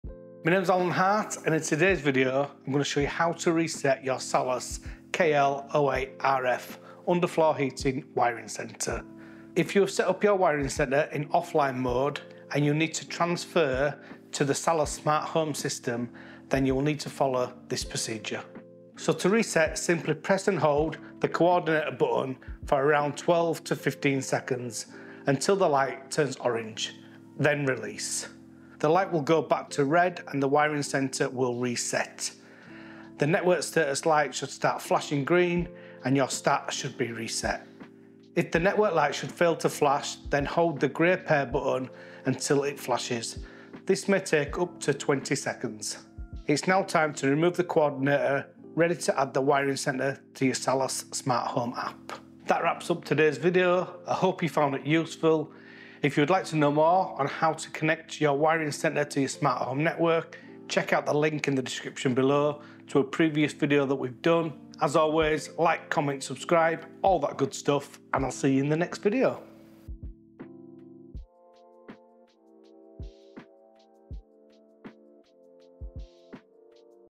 How To Reset Salus KL08RF Underfloor Heating Wiring Centre
Mee name’s Allen Hart and in today's video I'm going to show you how to reset your Salus KL08RF underfloor heating wiring centre. If you have set up your wiring centre in offline mode and you need to transfer to the Salus Smart Home system then you will need to follow this procedure. So to reset simply press and hold the coordinate button for around 12 to 15 seconds until the light turns orange then release. The light will go back to red and the wiring centre will reset. The network status light should start flashing green and your start should be reset. If the network light should fail to flash then hold the grey pair button until it flashes. This may take up to 20 seconds. It's now time to remove the coordinator ready to add the wiring centre to your Salus Smart Home app. That wraps up today's video. I hope you found it useful. If you would like to know more on how to connect your wiring centre to your Smart Home network check out the link in the description below to a previous video that we've done.